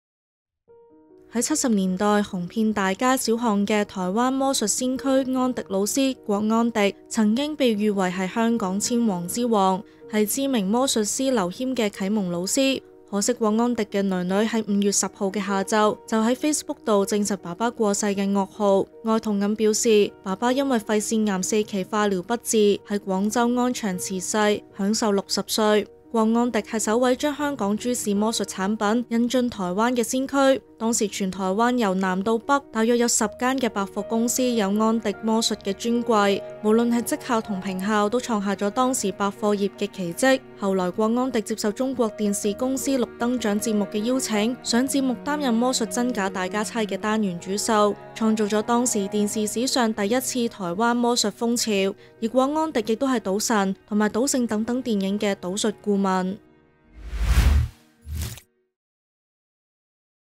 「香港千王」郭安迪肺癌病逝 為劉謙啟蒙老師 曾任《賭神》顧問
70年代紅遍大街小巷的台灣魔術先驅安迪老師（郭安迪），曾被譽為香港千王之王，也是知名魔術師劉謙、粘立人的啟蒙老師，驚傳因罹肺腺癌4期化療不治，於5月10日上午病逝，享壽60歲，女兒及粘立人悲慟於Facebook證實噩耗。

原文網址: 「香港千王」郭安迪肺癌病逝　為劉謙啟蒙老師　曾任《賭神》顧問 | 香港01